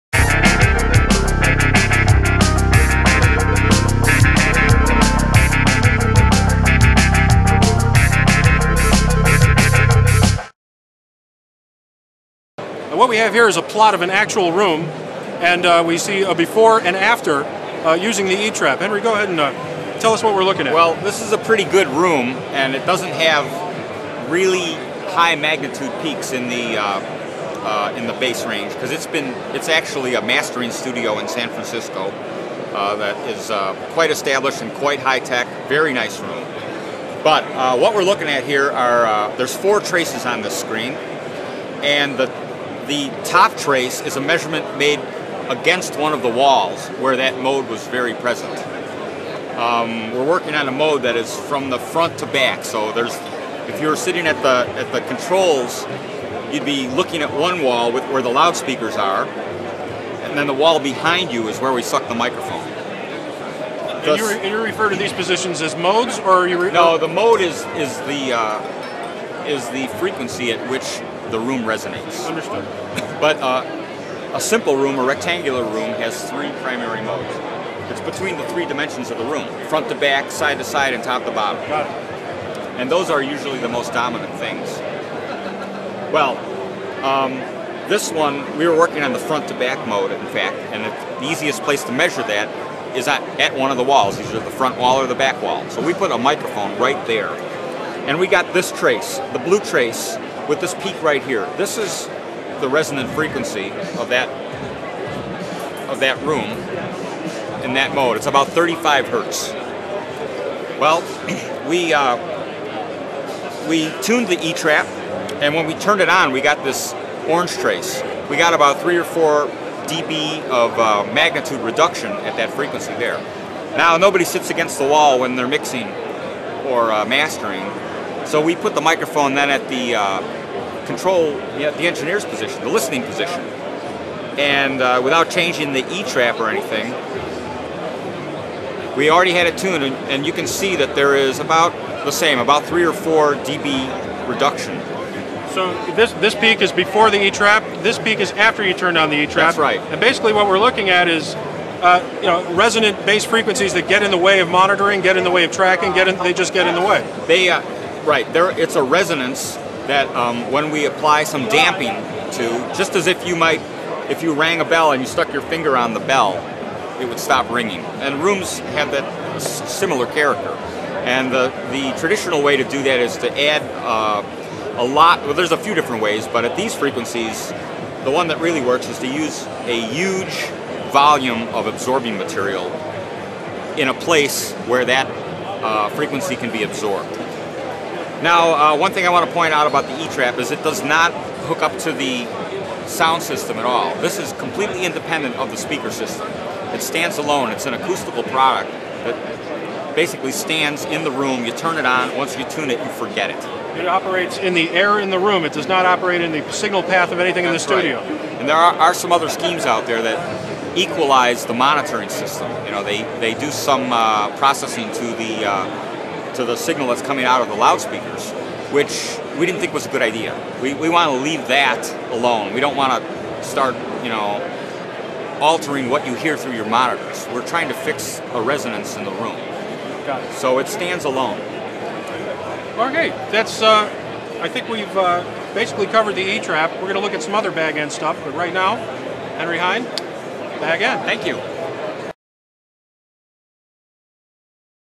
BAG END E-Trap Part Two - AES 2006
BAG END's E-Trap is
a tunable bass trap which attenuates frequencies down to 20hz. In part two of our look at the E-Trap,
we get a look at exactly how it works, including before-and-after results from an actual studio where the
E-Trap was used. This demo points out one excellent feature; this is not something you hook up
to your soundboard or monitoring system. It functions independently of your electronics to control
resonance in the room.